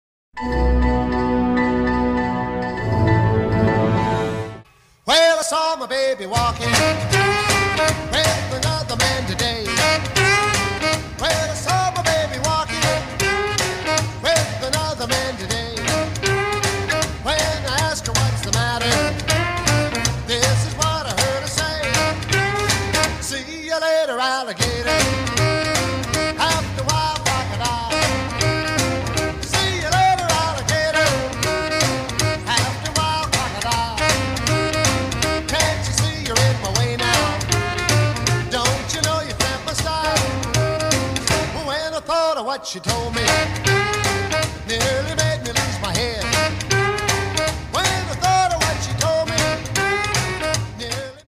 Alligator Lesson
Hello students! This trailer was made for our alligator weaving puppets!